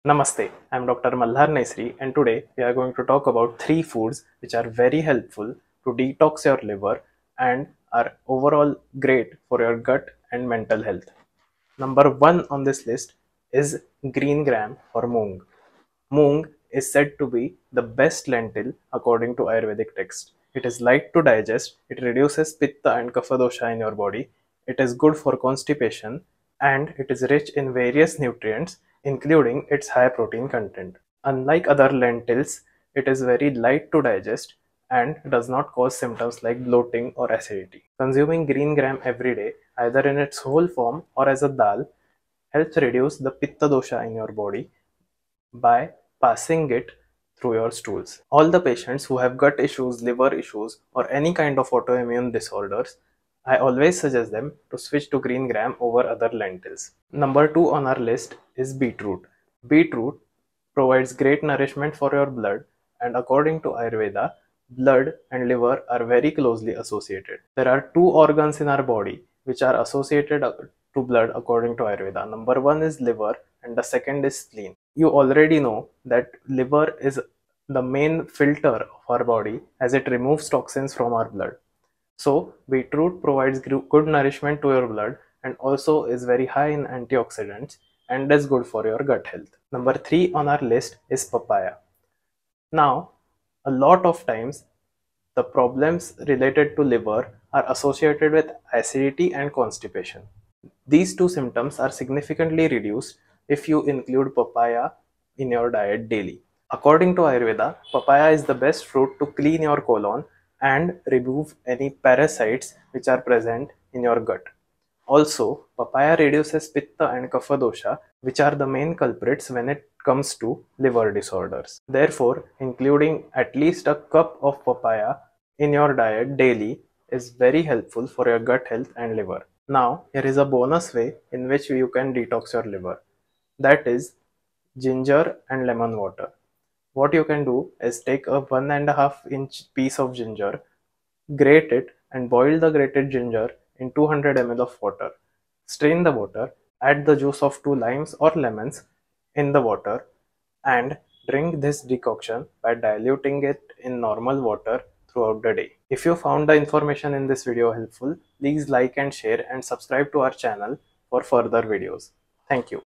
3 Superfoods to detox your liver!
Hello! I am Dr Malhar Nesari an Ayurveda practitioner from Pune India.

In this video I share 3 superfoods which can help you detox your liver (watch till the end for a bonus one!)

Please leave video suggestions in the comments below!